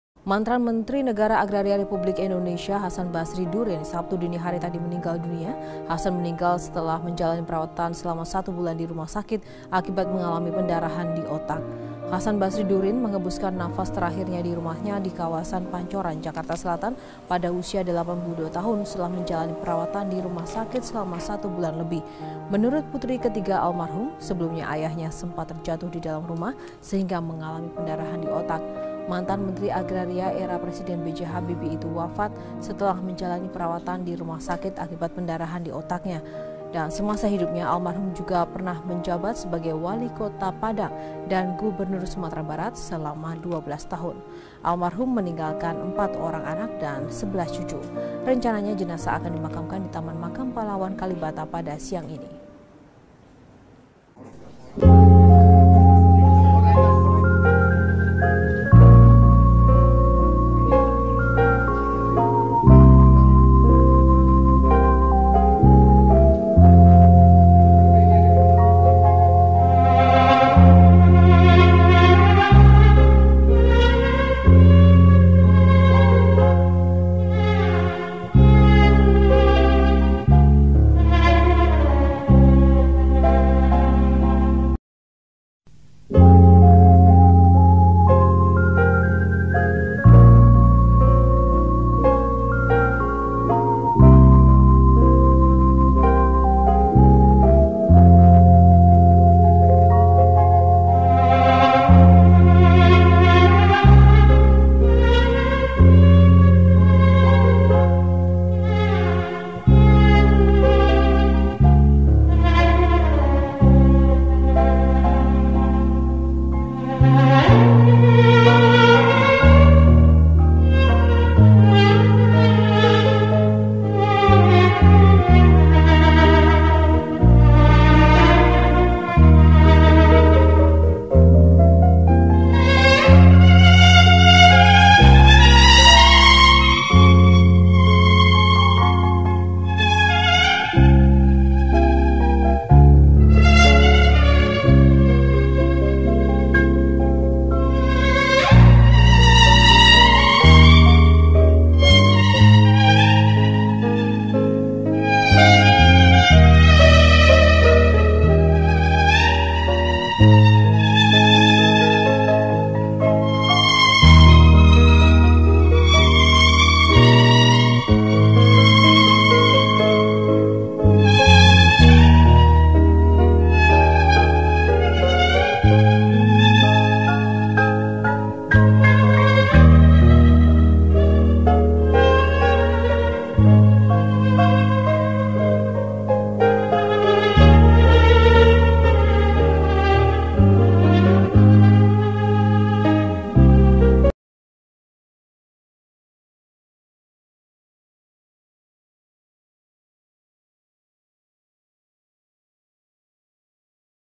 Hasan Basri Durin Wafat
Selamat Jalan Paktuo Hasan Basri Durin
Semoga Amal bakti beliau memudahkan kehidupan di alam baka.